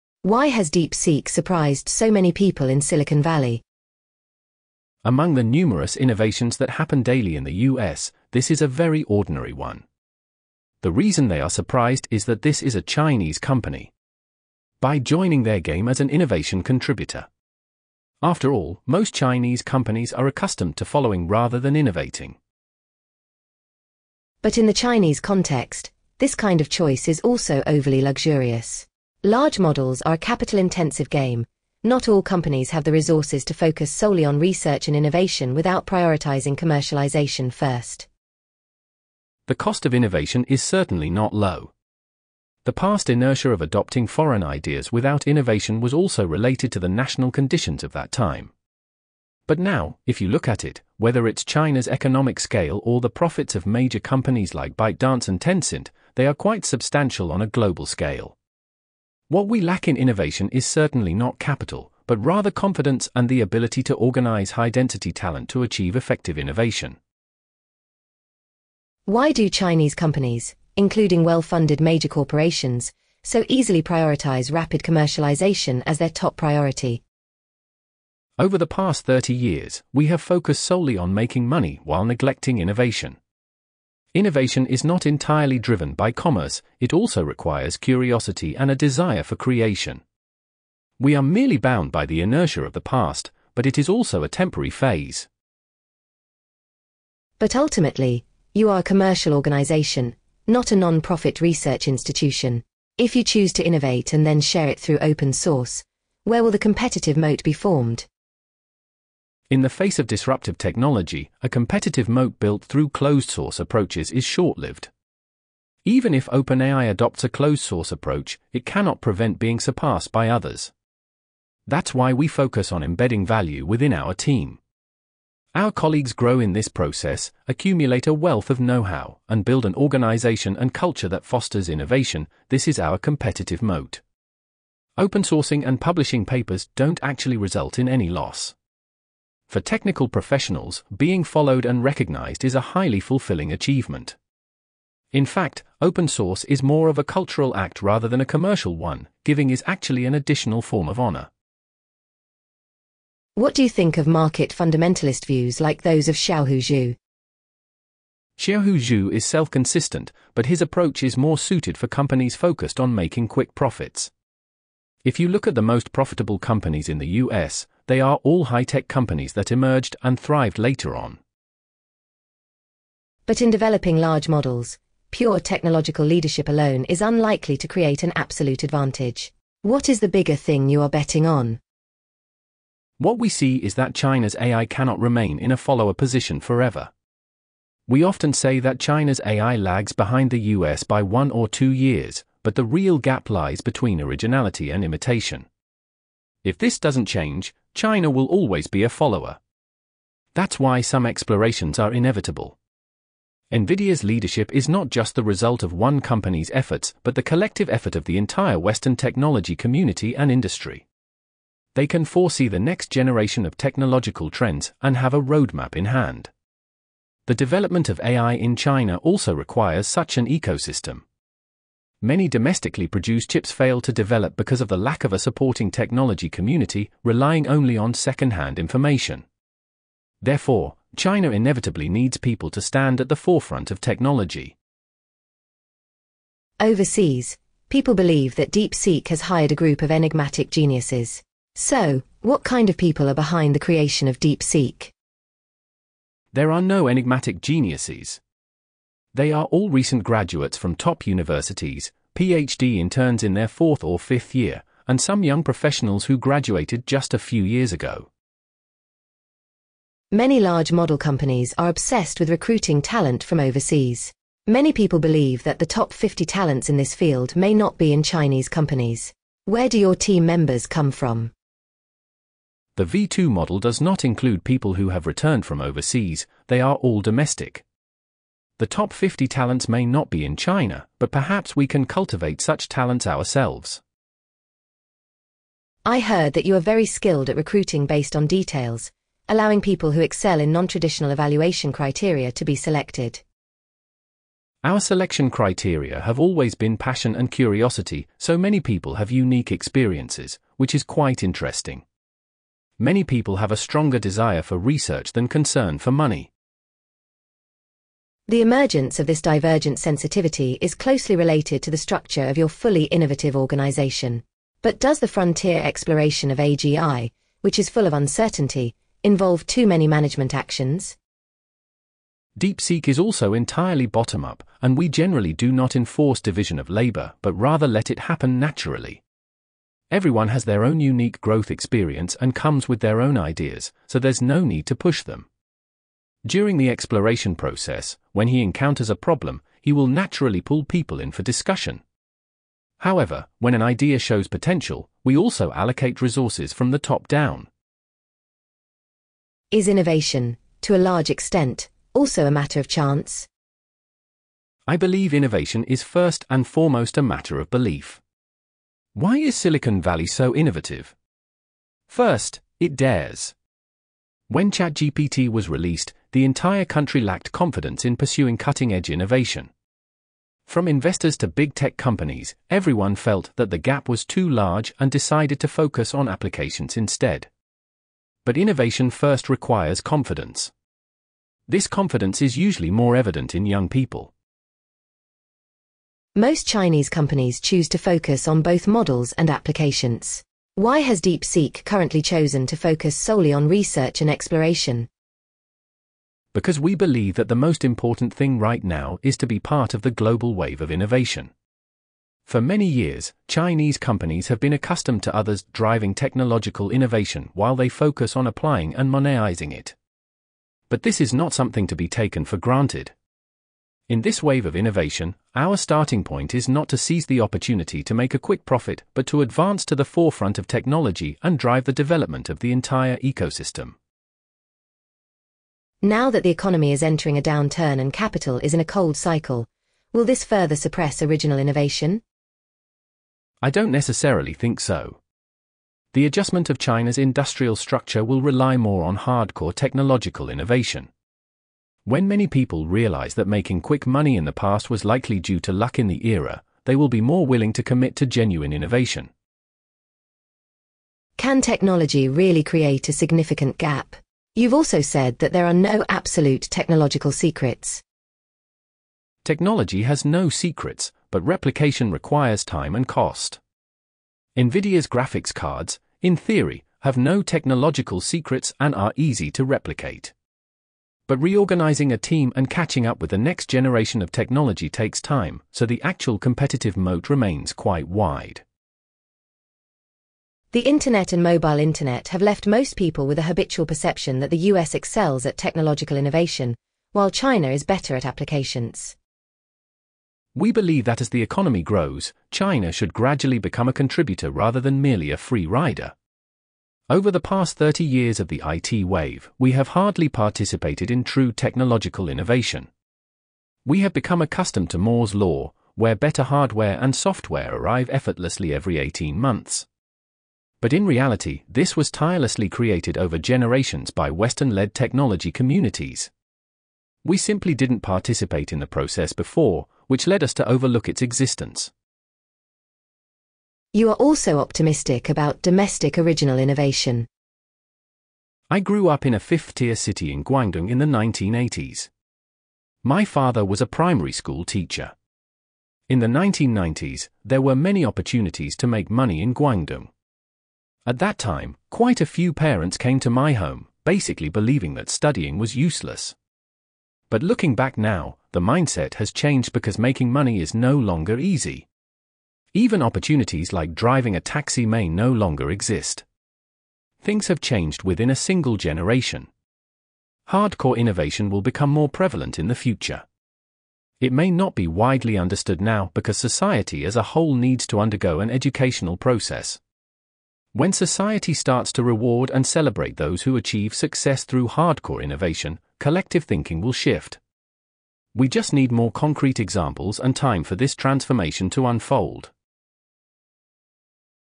Interview with DeepSeek's Founder (English Audio)
1. The audio of the interview is AI-synthesized (English). It is based on my previous YouTube video:
Interview with DeepSeek's Founder (DeepSeek创始人访谈)

2. The interview itself is based on a real conversation that took place in July 2024: